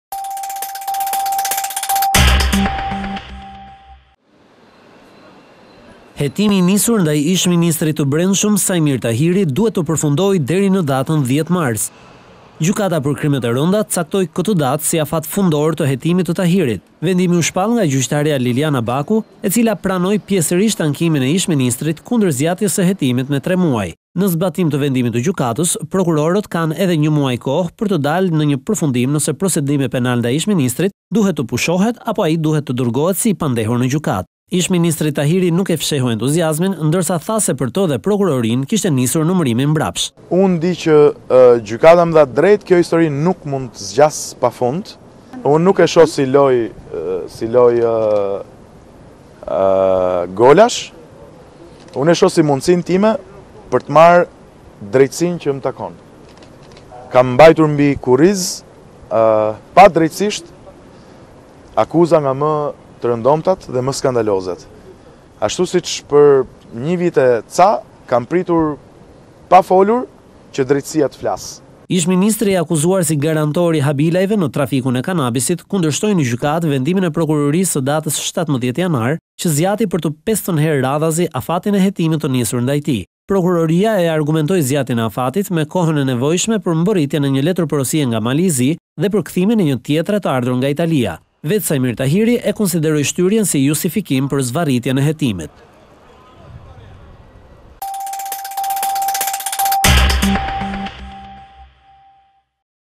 Dosja “Tahiri”, Gjykata rrezon Prokurorinë | ABC News Albania
Ish-ministri i Brendshëm Saimir Tahiri, ka fituar një tjetër betejë në luftën e tij me Prokurorinë për Krimet e Rënda. Gjykata pranoi ankimimin e bërë prej tij kundër zgjatjes me 3 muaj të hetimit, duke u lënë kohë prokurorëve deri në 10 Mars për ta dërguar ose jo dosjen për gjykim.

Hetimi i nisur ndaj ish-ministrit të brendshëm, Saimir Tahiri, duhet të përfundojë deri në datën 10 Mars. Gjykata për Krimet e Rënda caktoi këtë datë si afat fundor të hetimit të Tahirit. Vendimi u shpall nga gjyqtarja Liljana Baku e cila pranoi pjesërisht ankimin e ish-ministrit kundër zgjatjes së hetimit me tre muaj.  Në zbatim të vendimit të gjykatës, prokurorët kanë edhe një muaj kohë për të dalë me një përfundim nëse procedimi penal ndaj ish-ministrit duhet të pushohet, apo ai duhet të dërgohet si i pandehur në gjykatë. Ish-ministri nuk e fsheu entuziazmin, ndërsa tha se për të dhe prokurorinë kishte nisur numërimi mbrapsh.

Ish-ministri i akuzuar si garantor i “Habilajve” në trafikun e kanabisit kundërshtoi në gjykatë vendimin e Prokurorisë së datës 17 janar, që zgjati për të pestën herë radhazi afatin e hetimit të nisur ndaj tij. Prokuroria e argumentoi zgjatjen e afatit me kohën e nevojshme për mbërritjen e një letërporosie nga Mali i Zi dhe përkthimin e një tjetre të ardhur nga Italia. Ish-ministri e konsideroi shtyrjen si justifikim për zvarritjen e hetimit. /abcnews.al